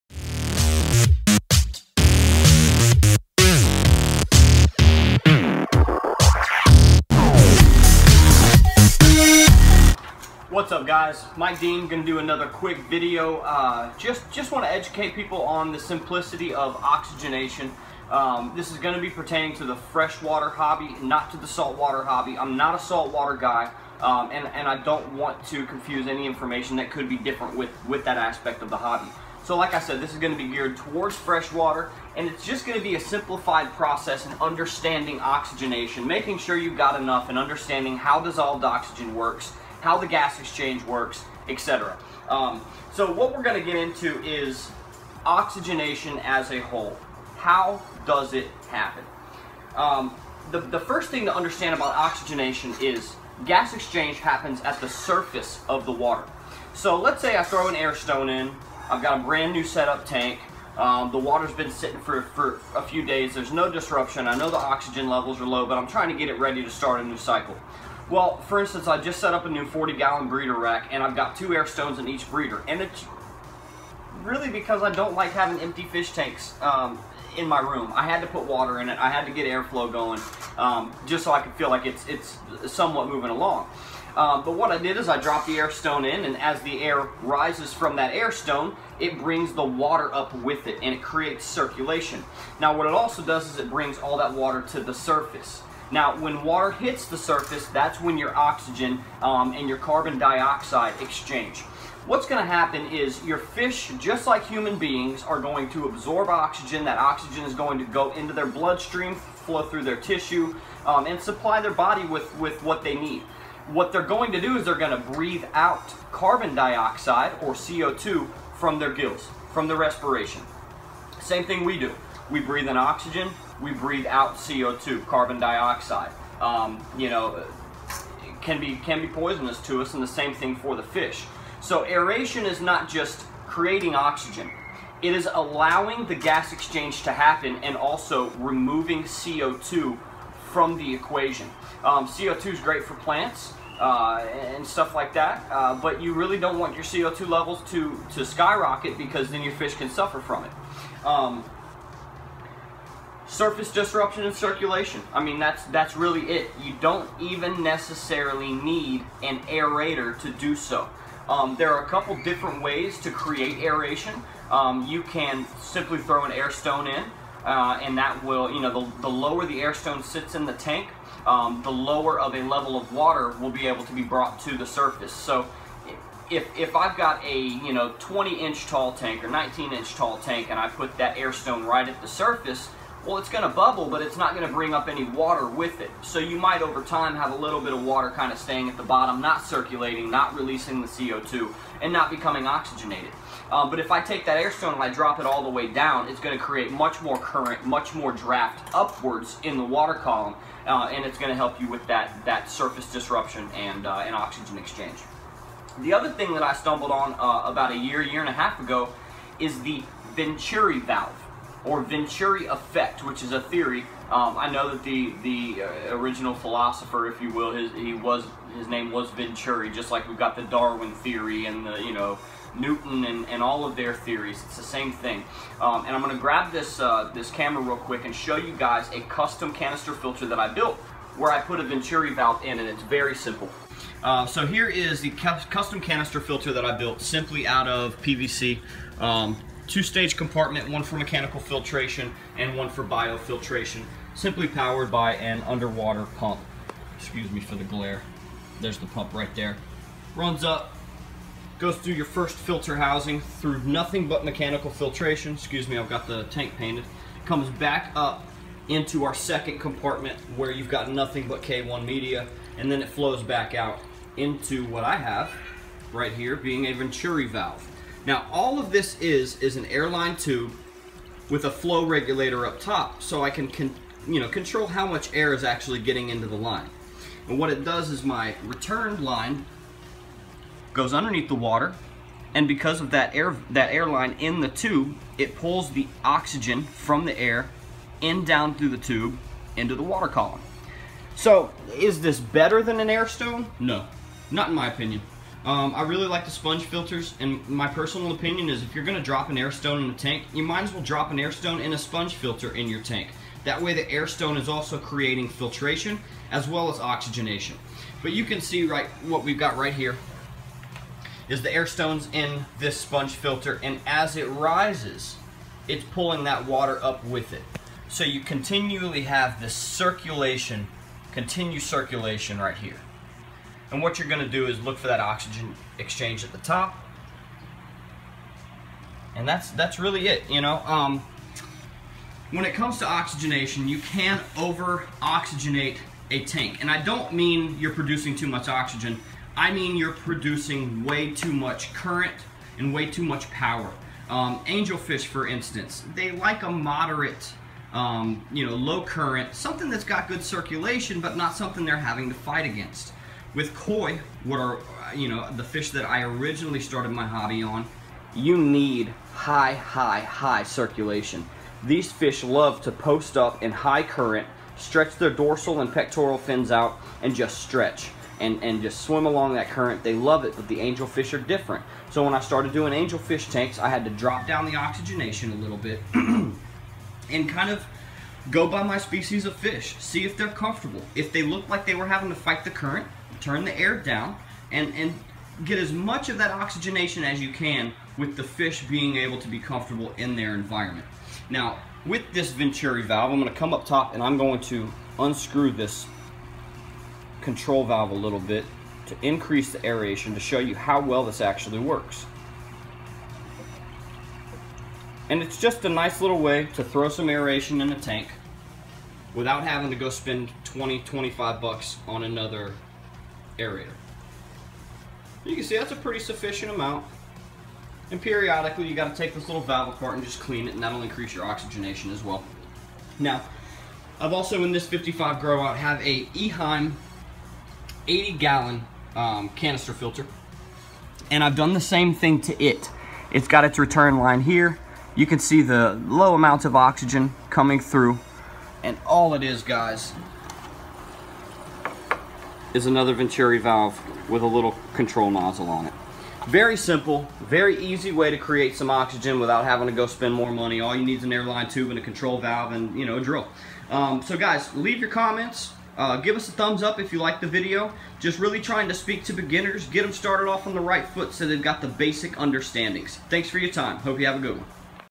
Oxygenation for beginners
Here I explain simple aspects of oxygenation/aeration for fish tank beginners. This doesn't cover injecting pure oxygen or any type of advanced water chemistry.